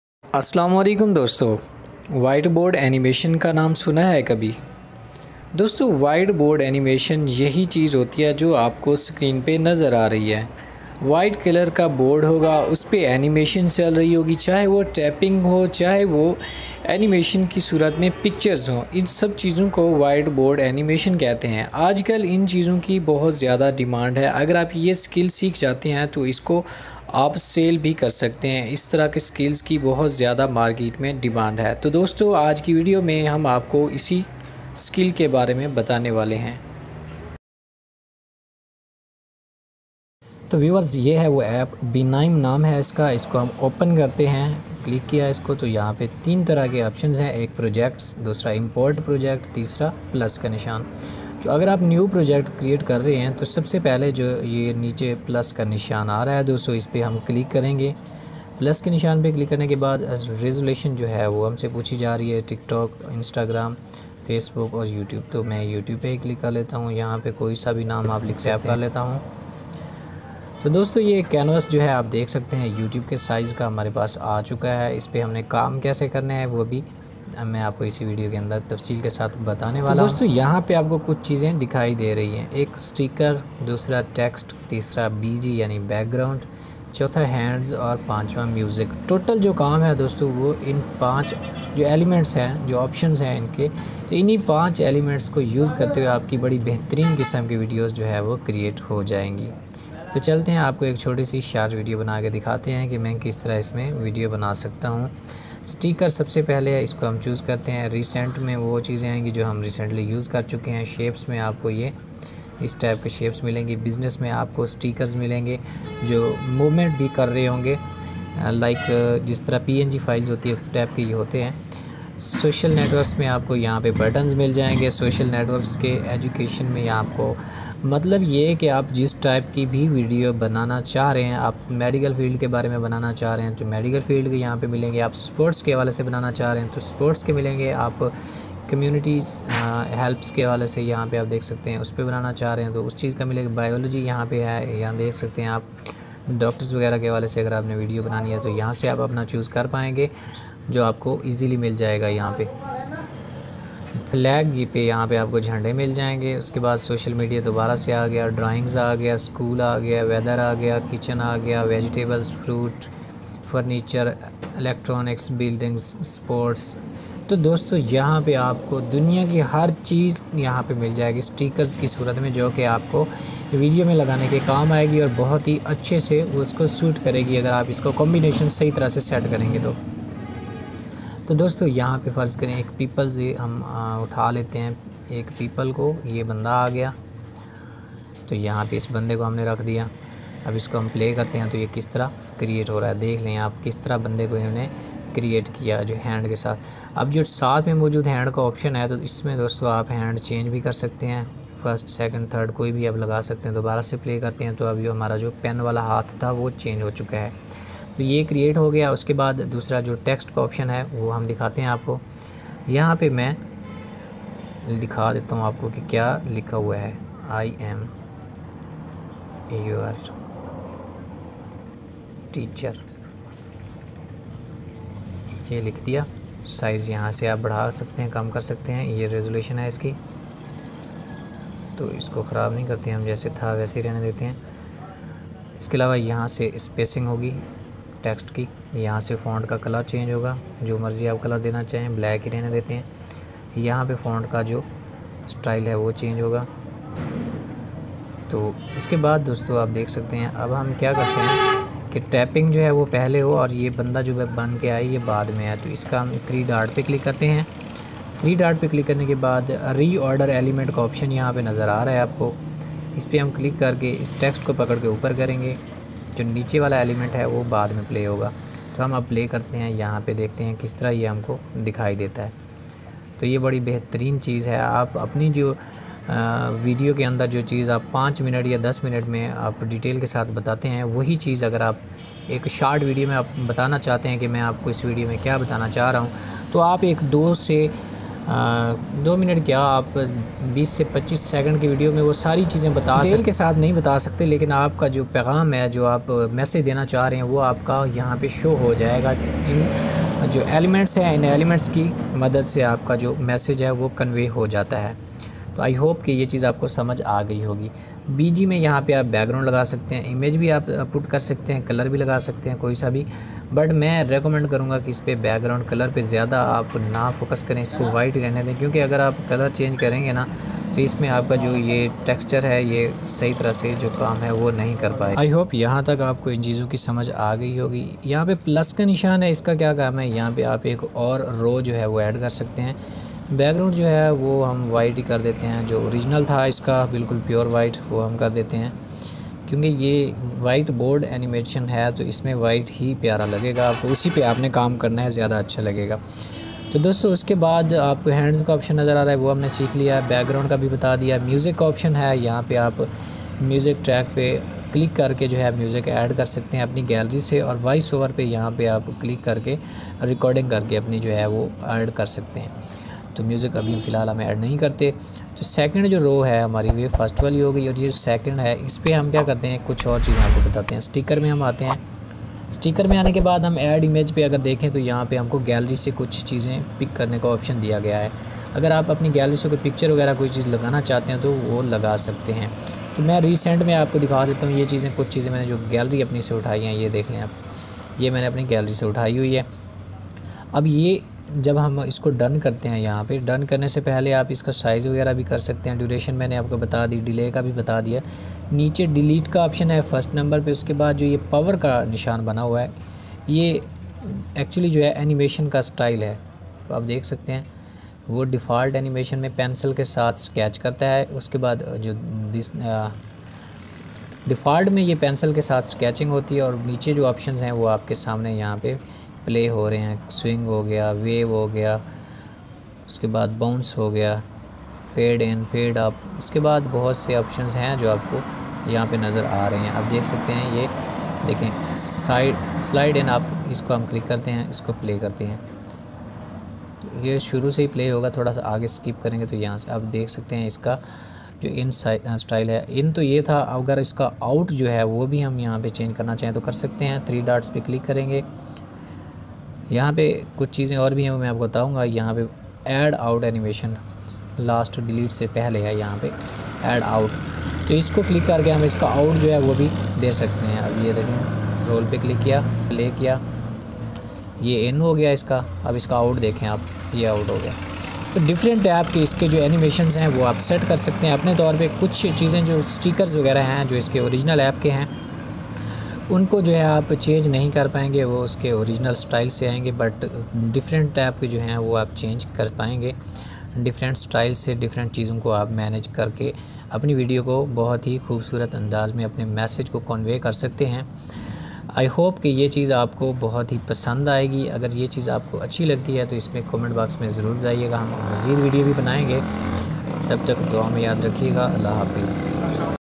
How to Create Whiteboard Animation in Mobile | White Board Animation Video Kaise Banaye
Learn This Video is About How to Create a Whiteboard Animation Video on Android. Make Your Favourite White Board Animation Video in Mobile with Few Simple Steps.

                          《FOLLOW US》

                     《VIDEO CREDITS》

► Voice Over: Muhammad Latif Khakh

► Editing: Muhammad Shahzad Khakh